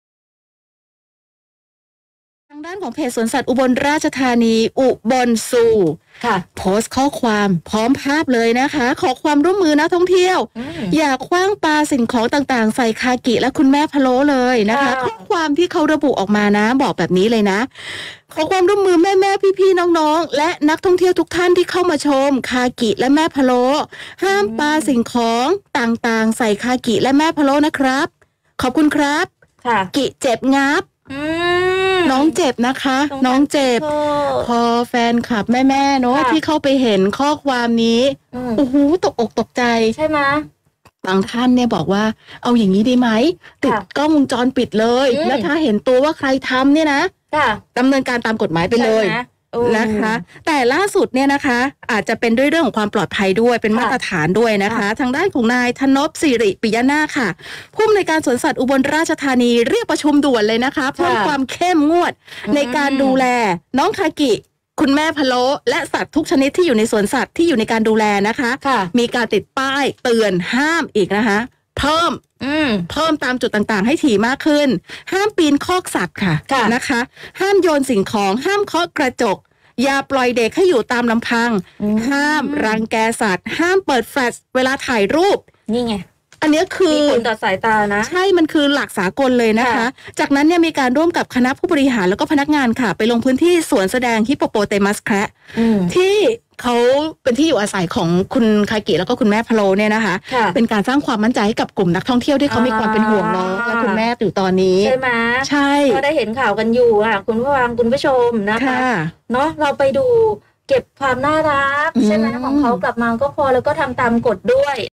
สวนสัตว์อุบลฯ ประชุมเข้ม หลังนักท่องเที่ยว ปาก้อนหินใส่ "น้องคากิ-แม่พะโล้"
16 ตุลาคม 2567 ผู้สื่อข่าว จ.อุบลราชธานี รายงานว่า หลังจากมีนักท่องเที่ยวโยนก้อนหินใส่ น้องคากิ ฮิปโปโปเตมัสแคระ วัย 8 เดือน ดาวเด่นแห่งสวนสัตว์อุบลราชธานี ซึ่งมีศักดิ์เป็นหลานของ น้องหมูเด้ง

เพจ สวนสัตว์อุบลราชธานี ubon zoo โพสต์ขอความร่วมมือนักท่องเที่ยวห้ามขว้างปาสิ่งของต่างๆ ใส่คากิและแม่พะโล้ โดยระบุข้อความว่า "ขอความร่วมมือแม่ๆ พี่ๆ น้องๆ และนักท่องเที่ยวทุกท่านที่เข้ามาชม คากิและแม่พะโล้ ห้ามขว้างปาสิ่งของต่างๆ ใส่คากิและแม่พะโล้นะครับ ขอบคุณครับ กิเจ็บงับบ"ขณะที่บรรดาแฟนคลับและคนรักสัตว์ต่างเรียกร้องให้มีมาตรการดำเนินการกับผู้กระทำผิดหรือดำเนินคดีตามกฎหมาย

ล่าสุด นายทนบ ศิริปิยนาค ผู้อำนวยการสวนสัตว์อุบลราชธานี เรียกประชุมเพิ่มความเข้มงวด พร้อมกำชับมาตรการการดูเเลน้องคากิและสัตว์ทุกชนิดในการดูแล โดยเพิ่มป้ายเตือน ห้ามปีนคอกสัตว์ ห้ามโยนสิ่งของ ห้ามเคาะกระจก ห้ามปล่อยเด็กอยู่ลำพัง ห้ามรังแกสัตว์ และห้ามเปิดแฟลช ในการถ่ายรูป จากนั้นได้ร่วมกับคณะผู้บริหาร และพนักงาน ลงพื้นที่ส่วนแสดงฮิปโปโปเตมัสแคระ เพื่อสร้างความมั่นใจให้กับนักท่องเที่ยวและเอฟซีน้องคากิ